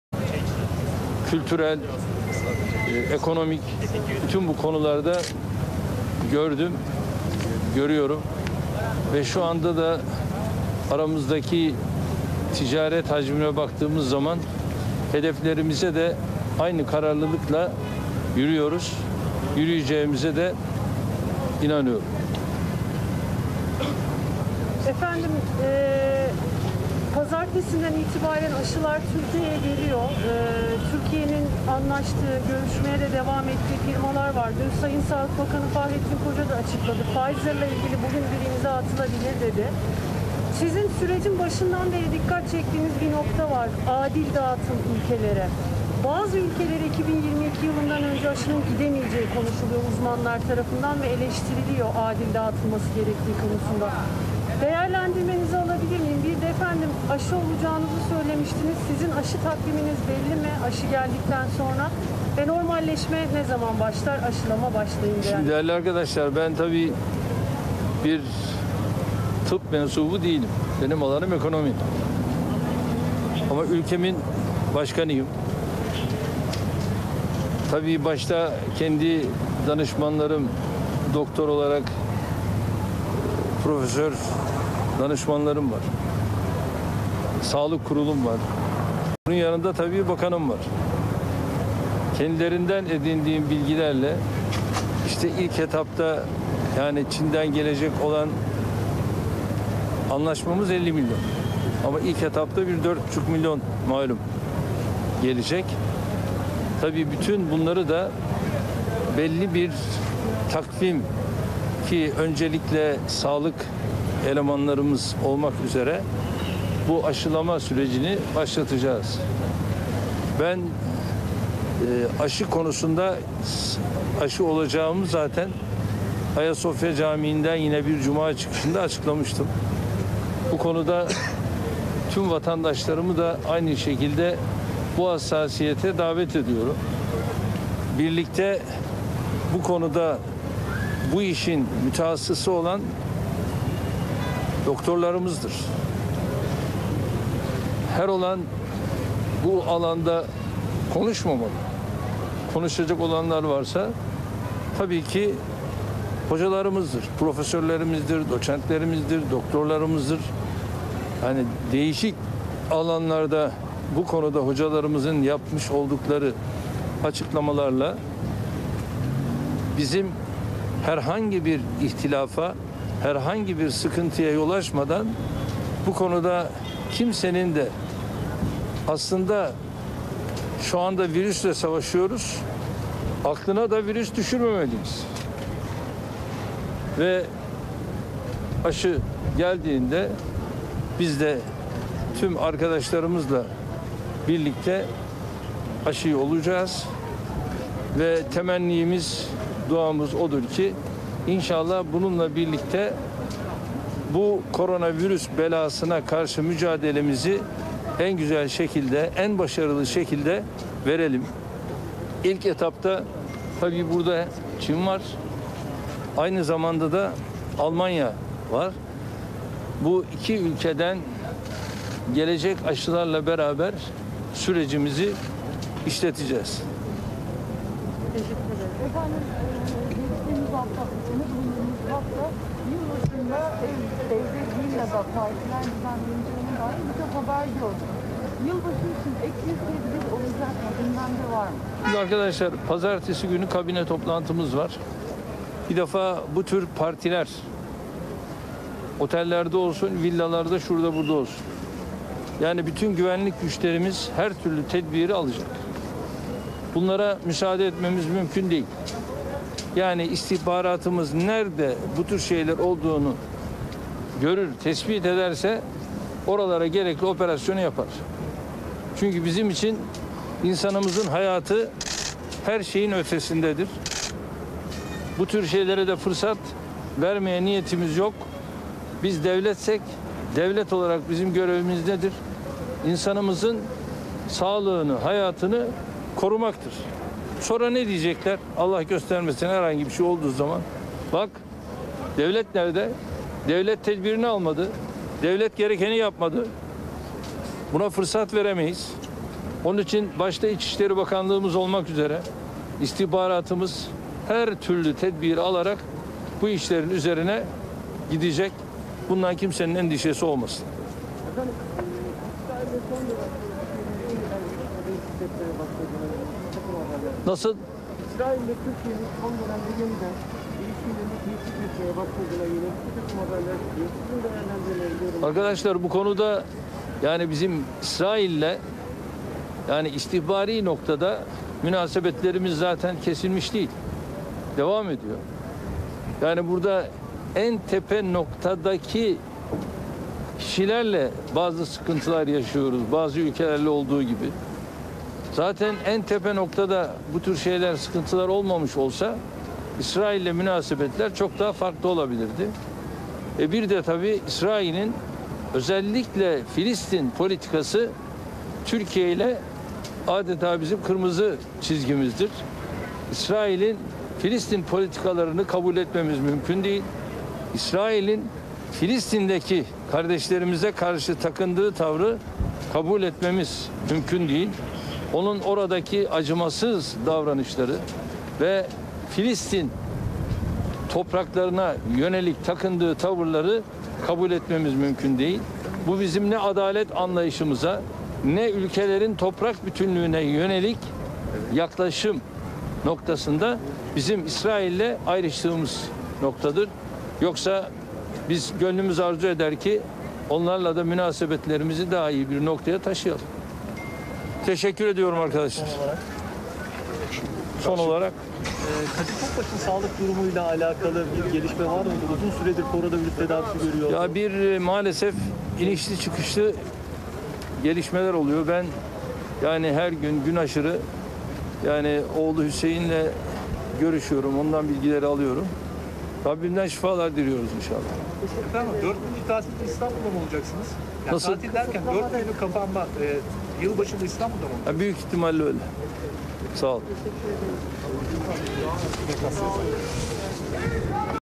SONDAKİKA Cumhurbaşkanı Erdoğan, yılbaşı kutlamalarına alınan tedbirleri açıklıyor
SONDAKİKA Cumhurbaşkanı Erdoğan: "Otellerde villalarda partilere izin verilmeyecek"
Çin aşısı Pazartesi geliyor...
İlk aşılama ne zaman başlayacak?

"Gücü Özgürlüğünde"
Doğru Haber, Dinamik Sunum, Kaliteli Program, Güçlü Yorum